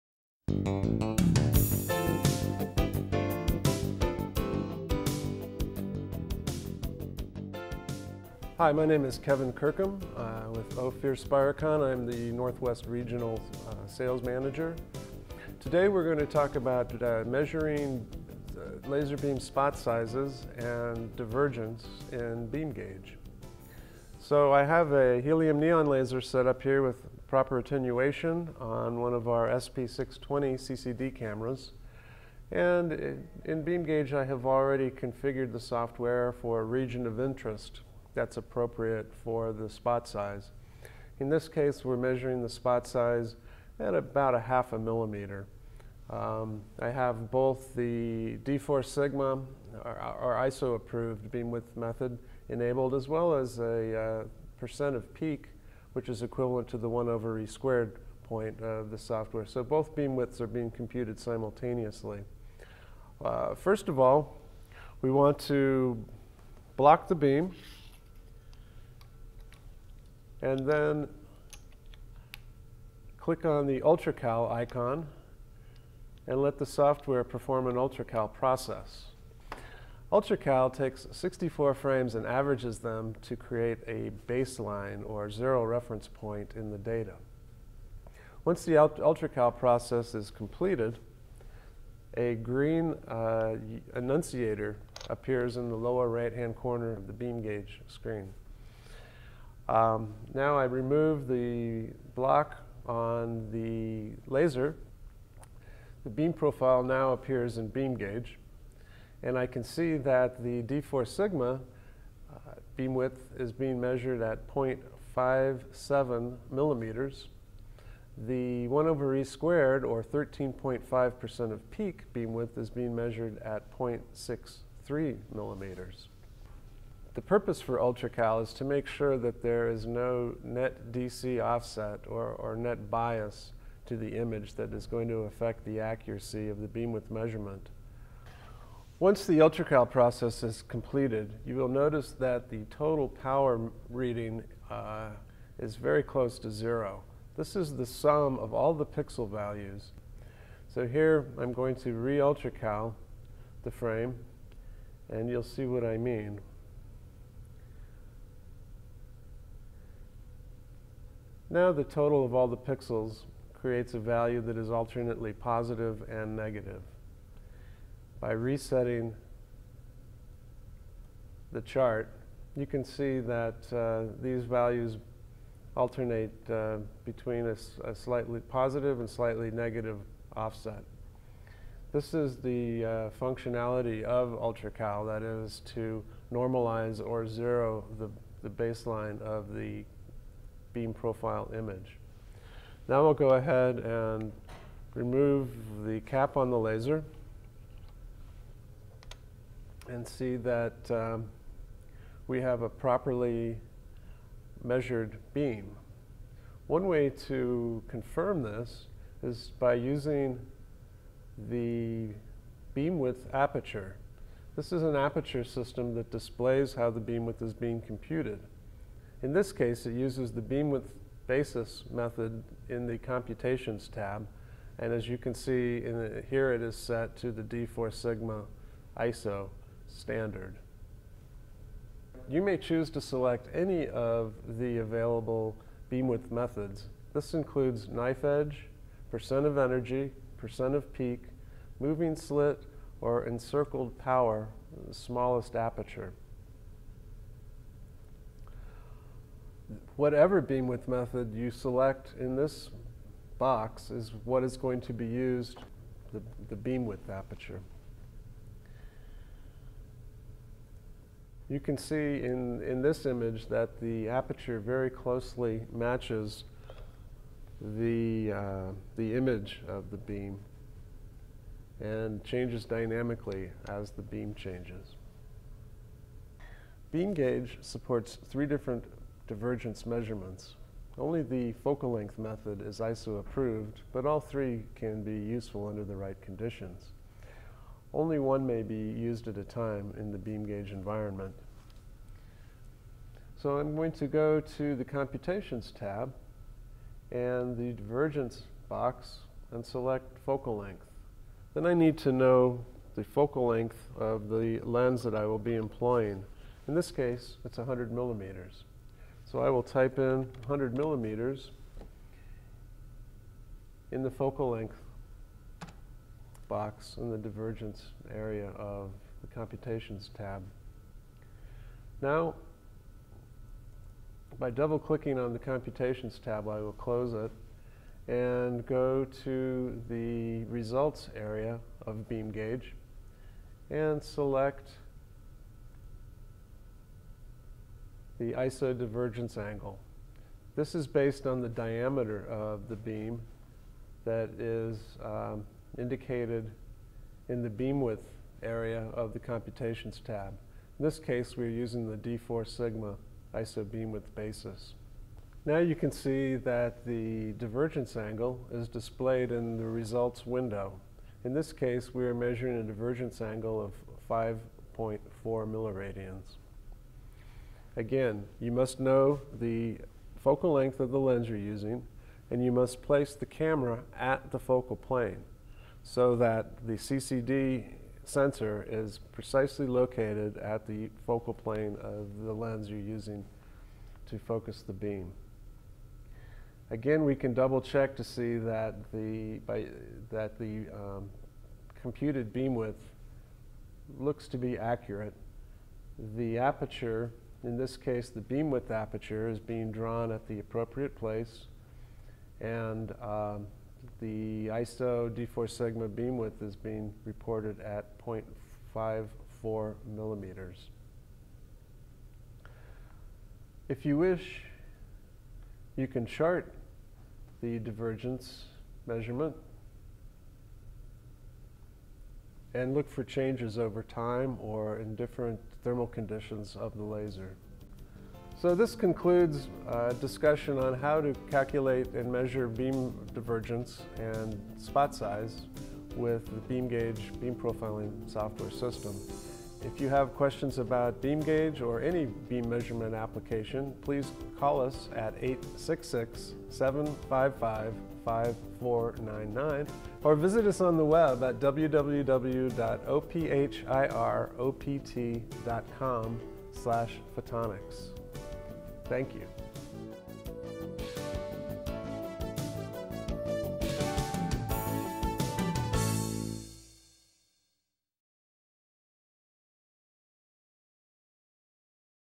BeamGage Tutorial: Spot Size and Divergence
Learn how to measure a lasers spot size and divergence in this BeamGage tutorial.